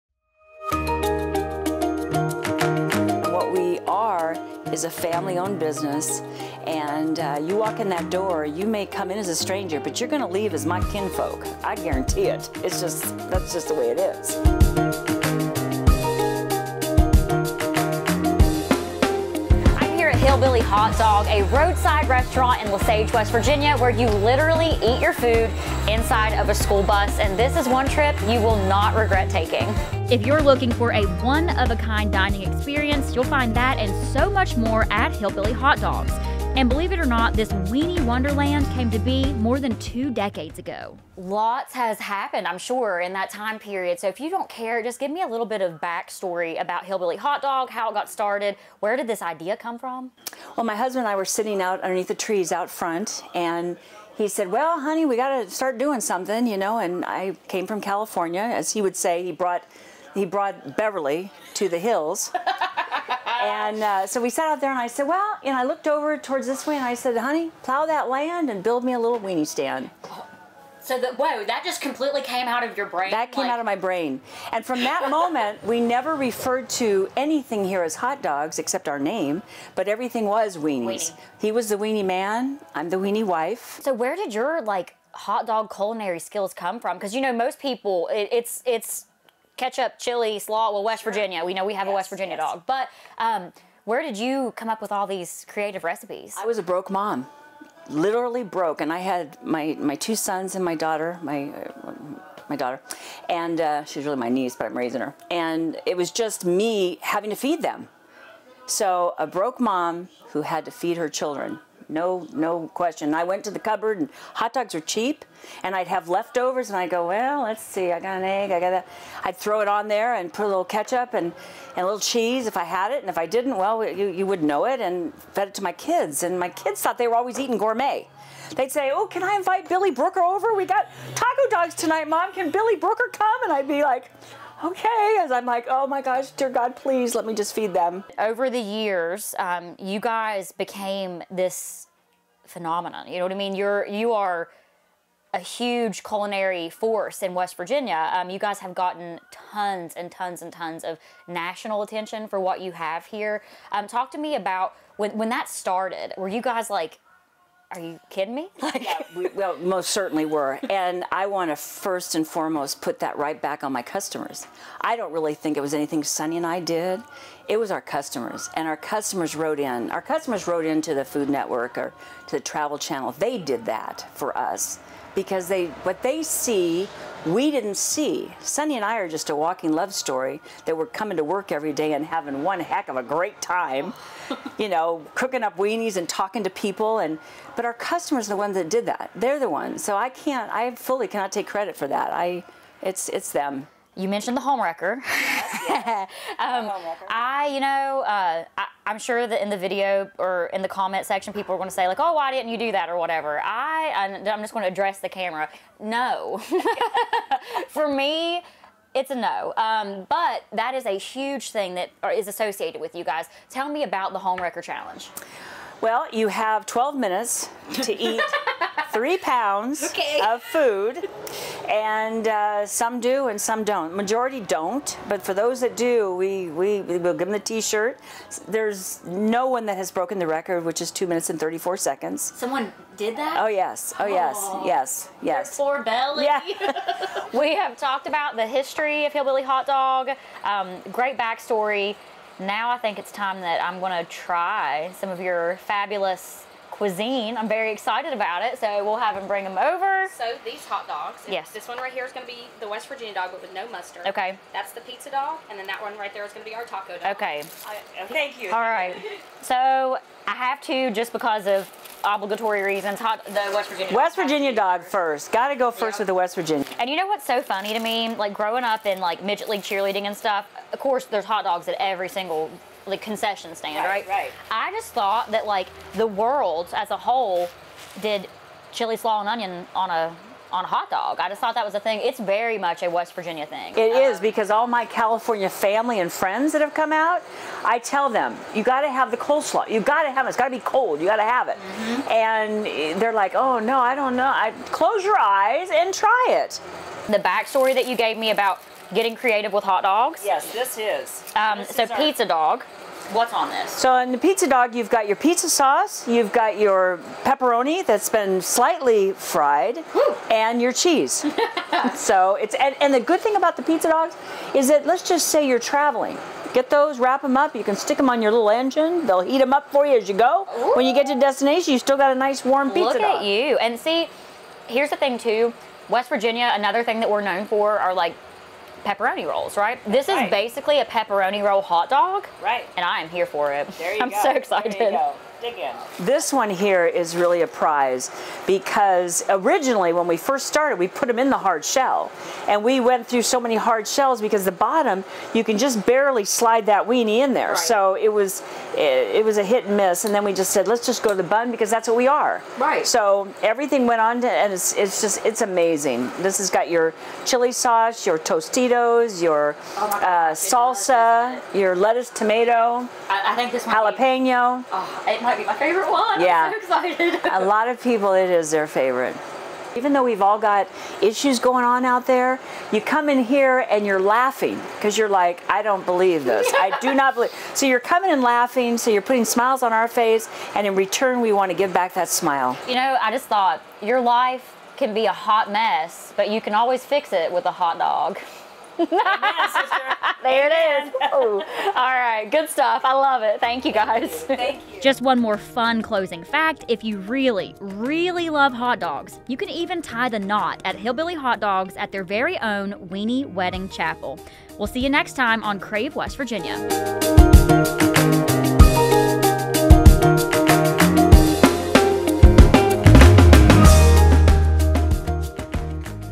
Hillbilly Hotdogs
Hillbilly Hotdogs in Lesage, West Virginia is a creative culinary destination that has captured national attention. Crave West Virginia talks to owner Sharie Knight about the inspiration behind her unique recipes and why she says her customers can be credited for the restaurant's success. From zany décor to mouthwatering hotdogs...you will not be disappointed by this one-of-a-kind roadside restaurant.

More info at: HillbillyHotdogs.com